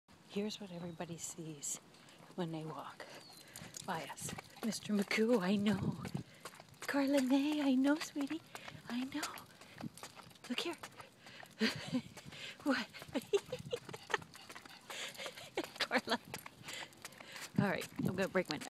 Two Lhasa Apso's showing BIG smiles
Mr MaGoo and Carla Mae are always smiling as they walk behind me, bringing big smiles to those that past us when we are walking.  Here is a glimpse as to what these folks see and experience as they pass us. Two big smiles, complete with a bounce or two in Mr MaGoo's step.